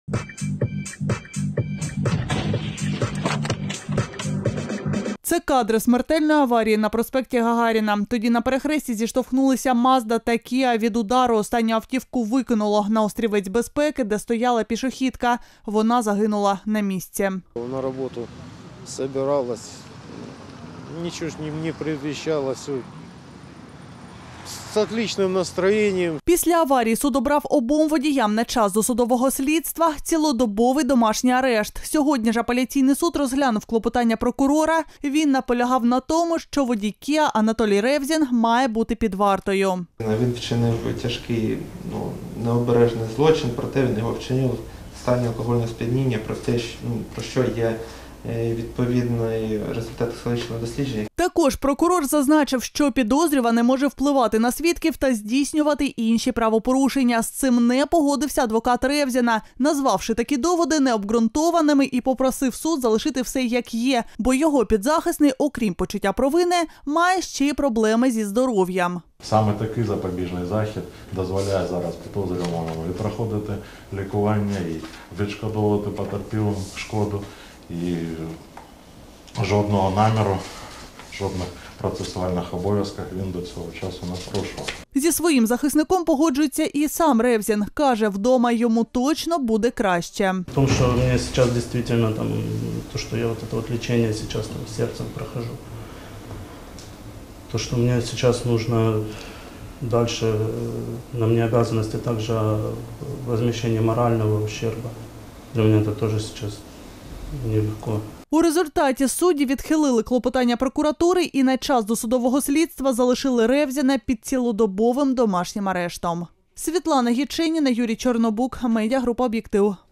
Учасника смертельної ДТП на Гагаріна залишили під домашнім арештом
Хворе серце та повне каяття. Сьогодні відбулася апеляція у справі смертельної аварії на проспекті Гагаріна, де на острівці безпеки під колесами КІА загинула пішохідка. Прокуратура наполягала на тому, що водій іномарки на час досудового слідства має повернутися під варту.